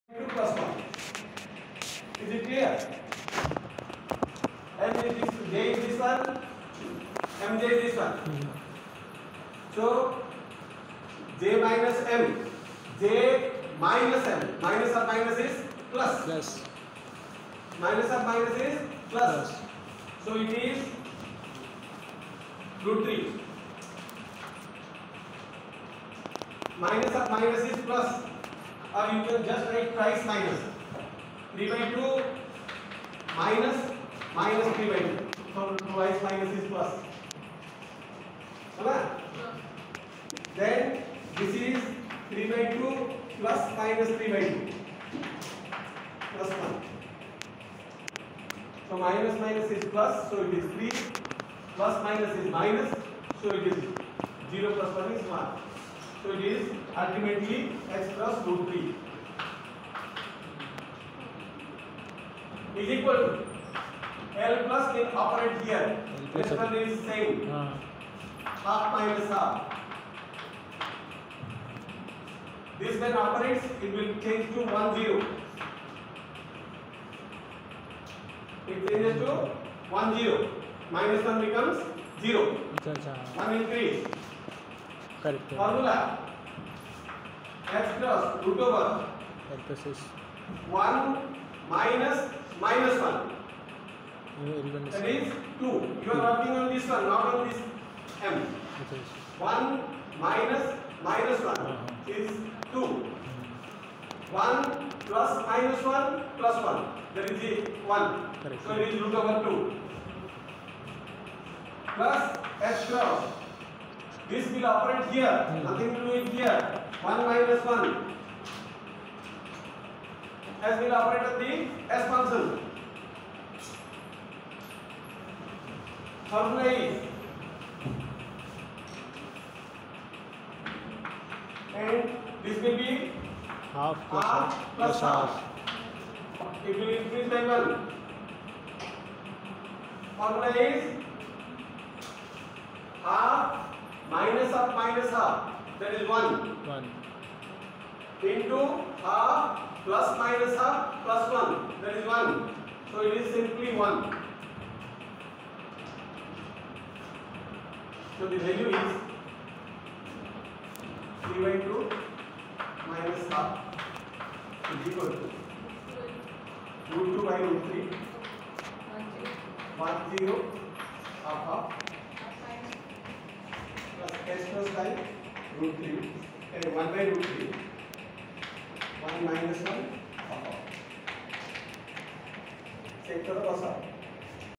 Department of Physics, ITER/ Shiksa 'O' Anusandhan University Bhubaneswar
The teacher - Professor PK Mohapatra.
Topic - Eigen State of two electron atoms
Subject - Quantum Mechanics
Department - Msc Physics